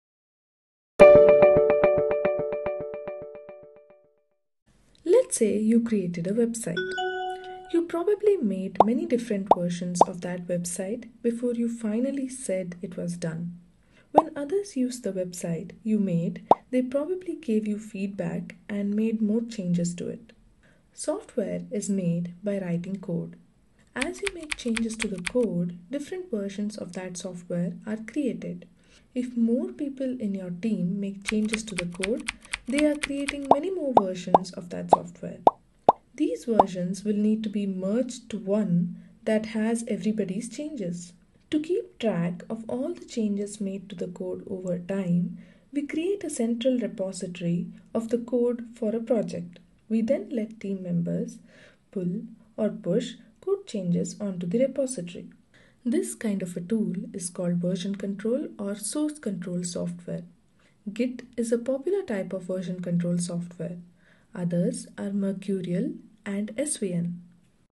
What is git? (in 1 minute)
This is short video about what is git and why we need it.

Anatomy of a filename: Chaz Hutton(@instachaz)
Peeps: OpenPeeps